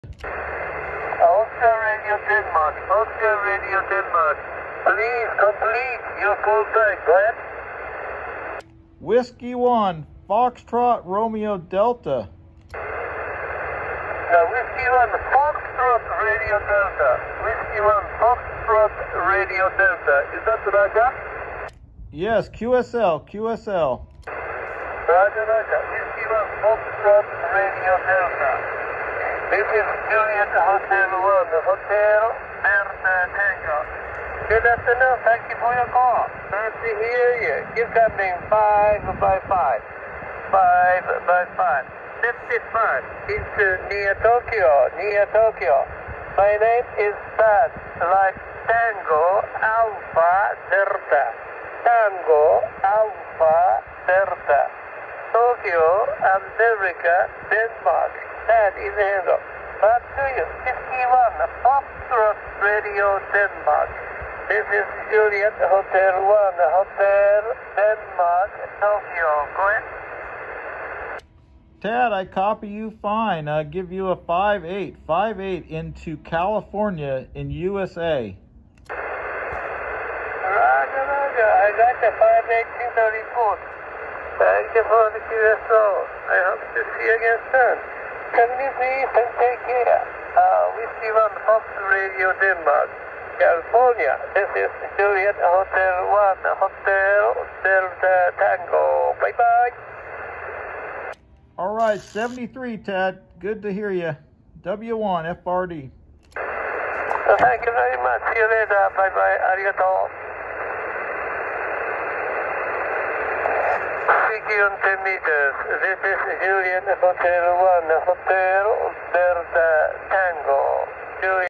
DX to Japan on my FT-817
Maldol AH-C28 base loading coil and Maldol stainless steel telescopic element for antenna. MX-P50M 45 watt amplifier. Yaesu FT-817 on battery power.